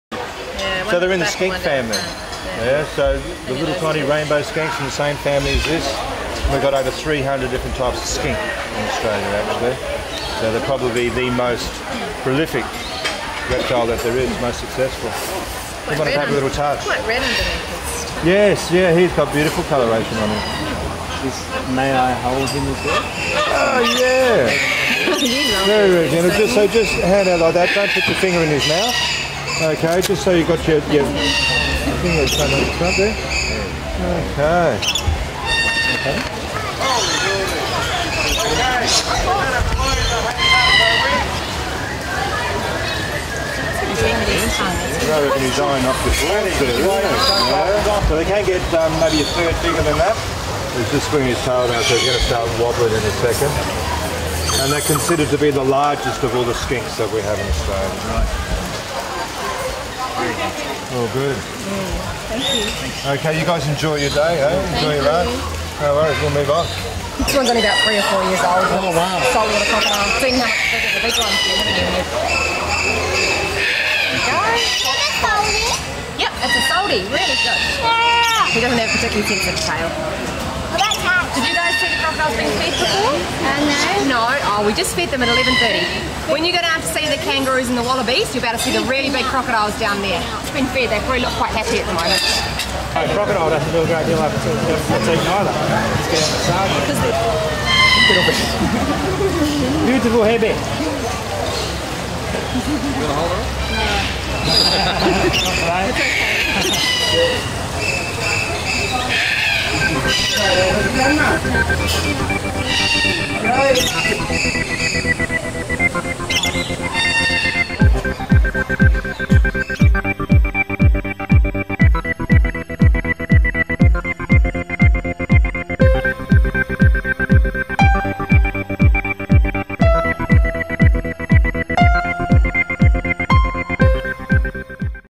Wildlife up close & personal
The Wildlife Habitat is located in Port Douglas far north Queensland.  It is one of the region's main attractions featuring close encounters with normally hard to spot local wildlife.

Subject to animal welfare considerations The Wildlife Habitat is able to offer close up & personal experiences to its visitors.  The wet tropics region of Australian, far north Queensland, is home to many unique animals, animals not found elsewhere.  This short video shows some of the experiences available during Australia Day 2012.

Much of the wildlife within the facility are essentially free roaming.  All of the animal carers & their helpers are passionate about wildlife & provide wonderful conduits for visitors to, occasionally, handle certain animals & enthusiastically deliver stories about their charges.  The Wildlife Habitat does have a programme of animal care & breeding.

Whether a wildlife enthusiast or simply interested The Wildlife habitat provides a wonderful day out for all ages.  or speak to your travel agent.

Featured within the short video is a Blue-tongued Skink (Lizard), a young Estuarine Crocodile, a mature Amethystine (Carpet) Python & a Koala.

Video:  12-001-australia-day-v1.2-HD720i-5M.mp4